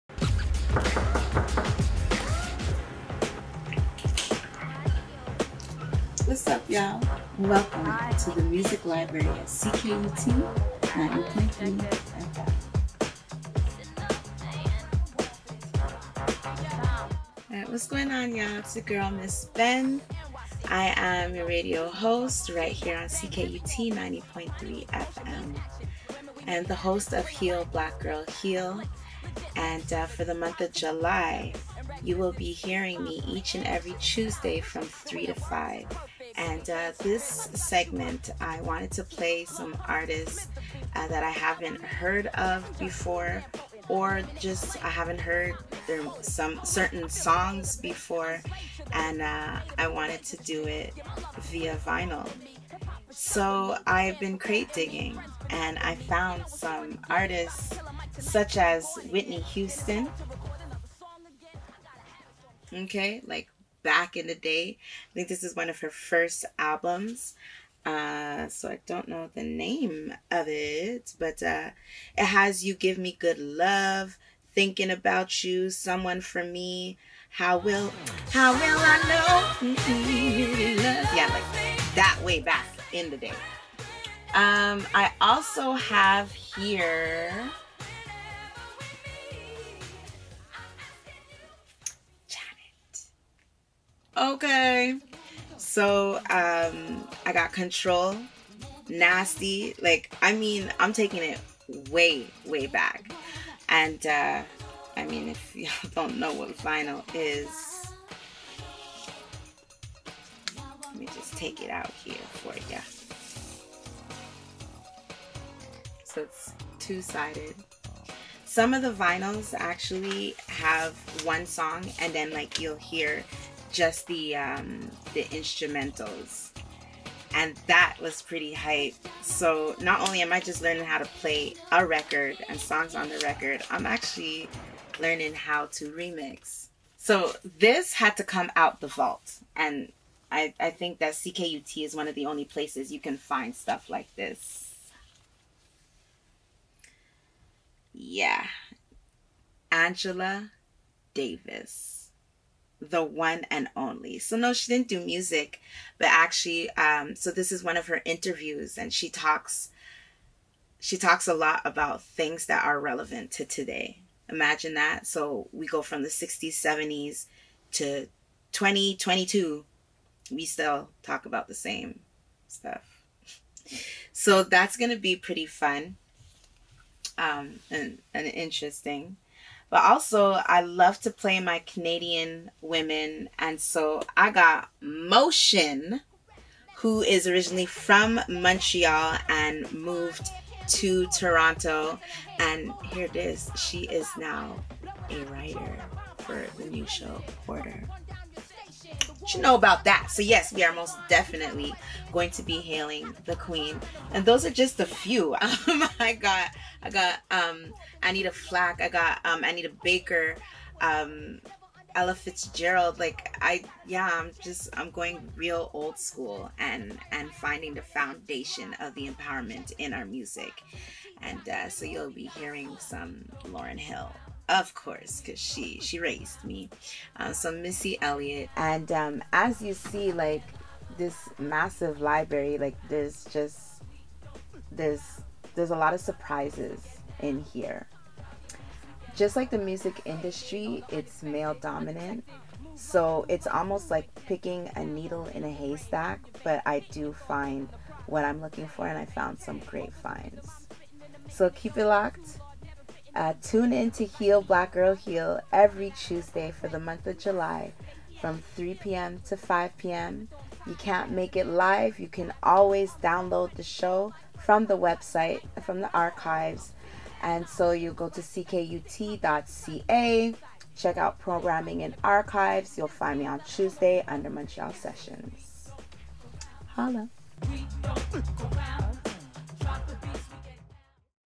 CKUT Picks - Ms. Benn
Hosting the second instalment of CKUT Picks, it's Ms. Benn! This July, she is hosting her show "Heal Black Girl, Heal" on The Montreal Sessions every Tuesday from 3-5pm! In this episode, Ms. Benn talks about how she prepared the music for her show - by crate digging in our archives!